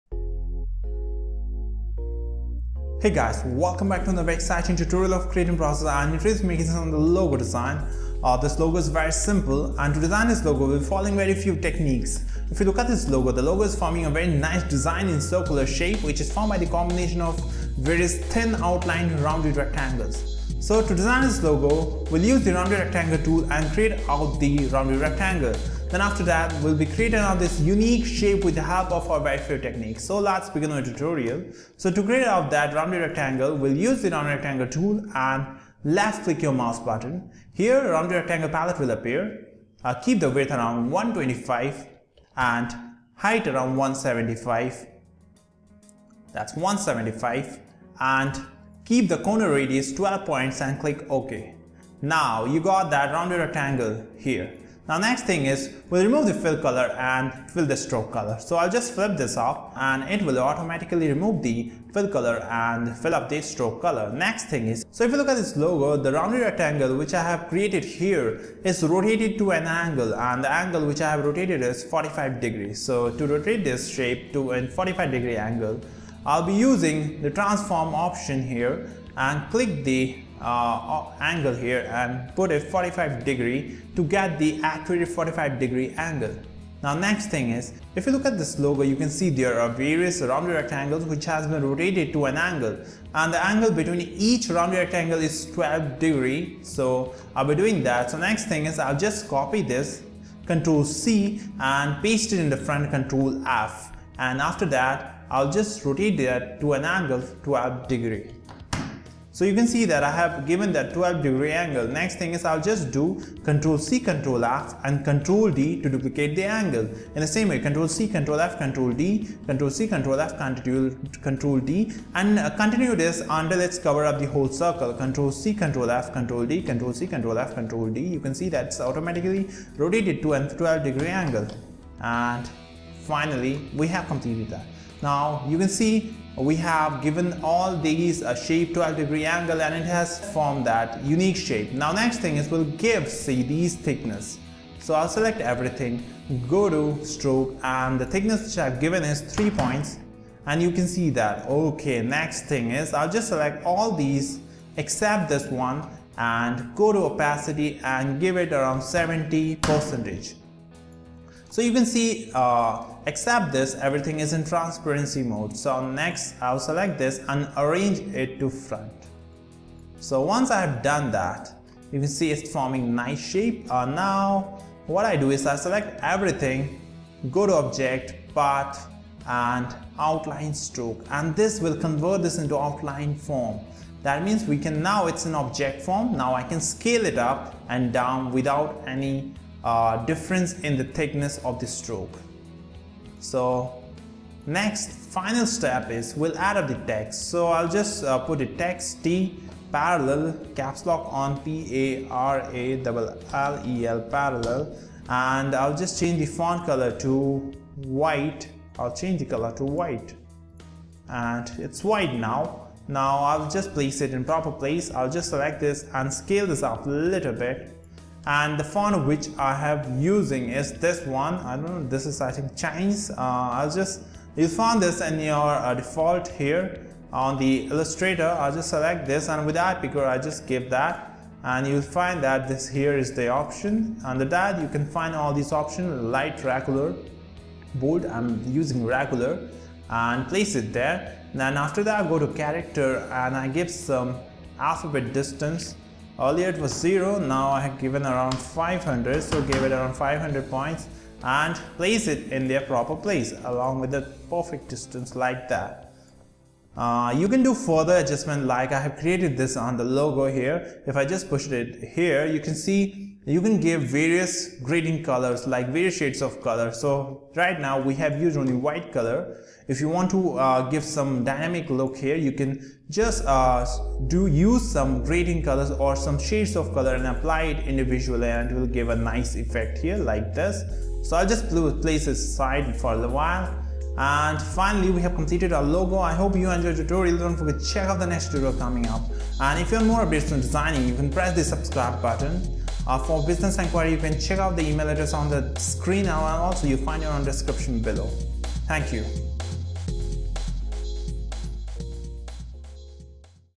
Professional Logo Design - Adobe Illustrator cc (PARALLEL)
Here is another new logo designing, This logo very simple. To design this logo , we will be following very few and easy techniques. The logo is forming very nice design in circular shape which is formed the combination of various thin outlined rounded rectangle.
So design the this logo we use the rounded rectangle tool and create out the rectangle. Then we will create out the unique shape using very cool techniques. We will understand the use of transform tool, angles, duplication etc.
Check out the whole designing process in this video.

▲Gumroad Store!

▲Creatnprocess Channel!